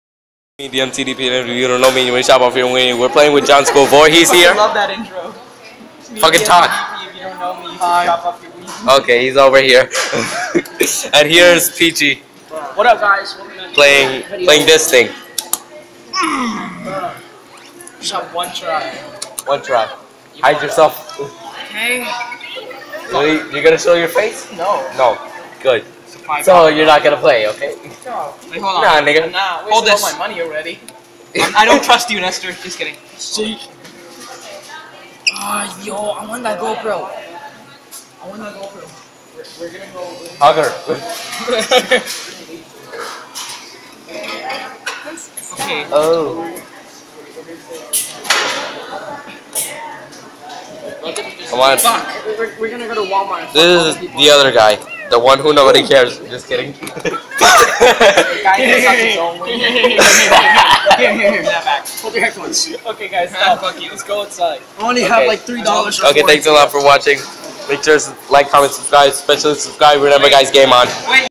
Some fun with friends. (ft. Pichi Vlogs, John Vorhees and Joseph Crespo)
We saw assassin's creed
Its dope!!!